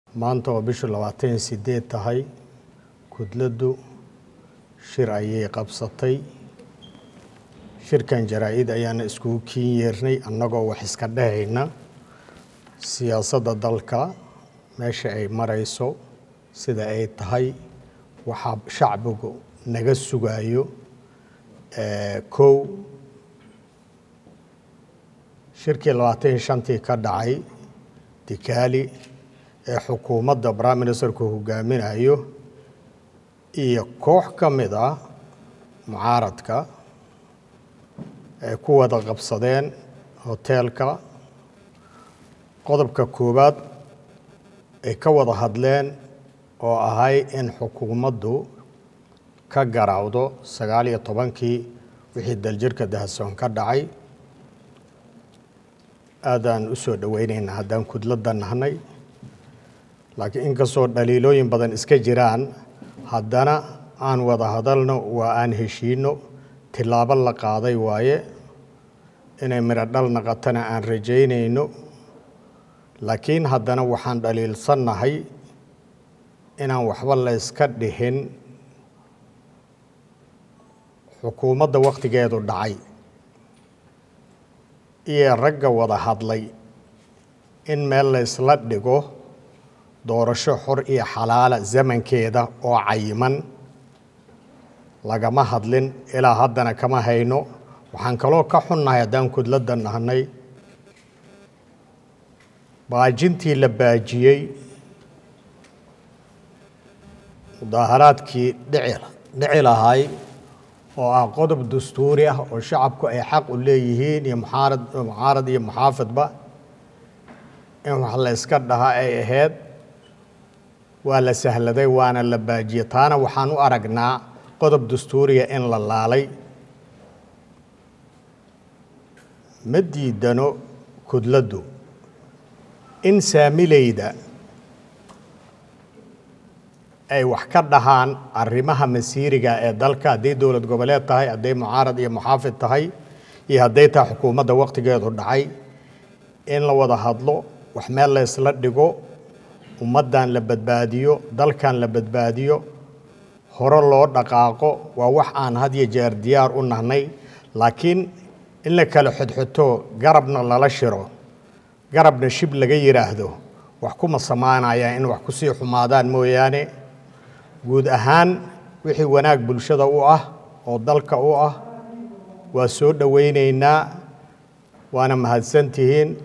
“Waxaan ka xunanahay in la baajiyo banaanyaxdadii lagu raadinayay xaqa doorashooyinka”
Dr.Maxamed Issak “Waxaan ka xunanahay in la baajiyo banaanyaxdadii lagu raadinayay xaqa doorashooyinka”